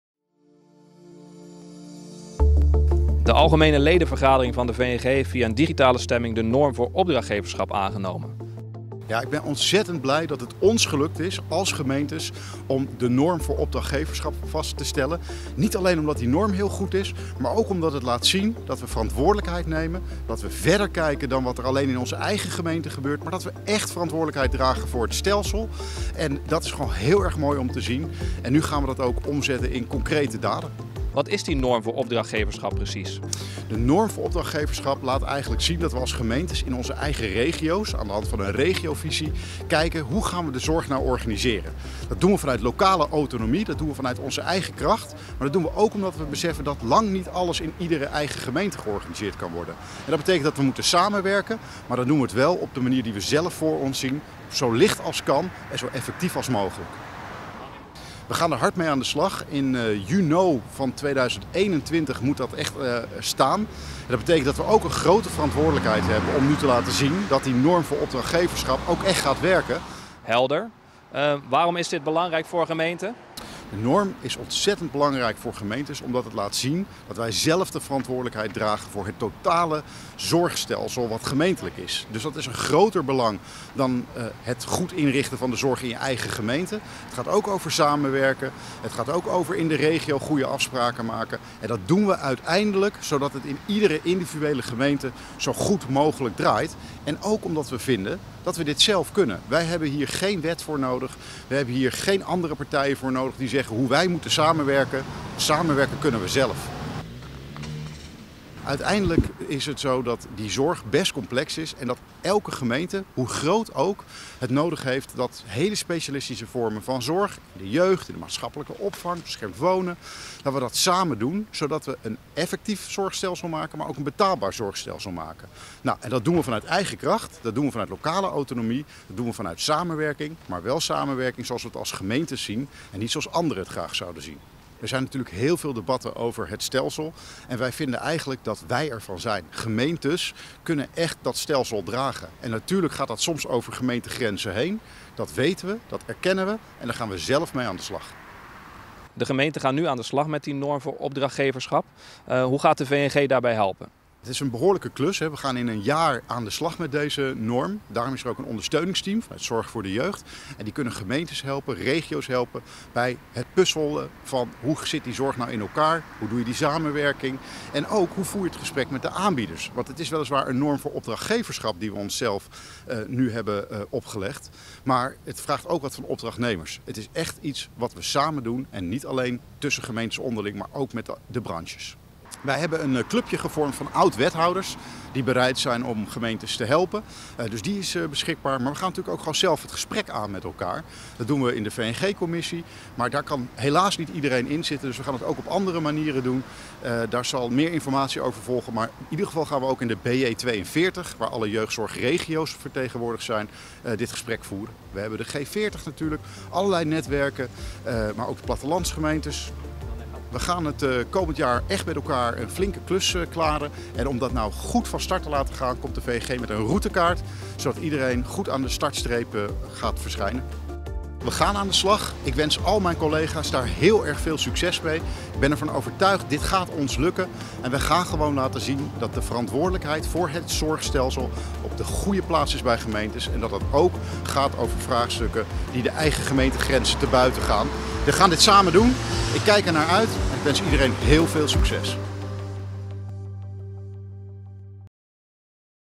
Aan de slag met de Norm voor Opdrachtgeverschap
In de ledenraadpleging is de Norm voor Opdrachtgeverschap aangenomen. Met deze norm spreken gemeenten zich collectief uit dat en hoe ze willen samenwerken. Het komende jaar gebruiken we om de norm te vertalen naar de regionale en lokale context.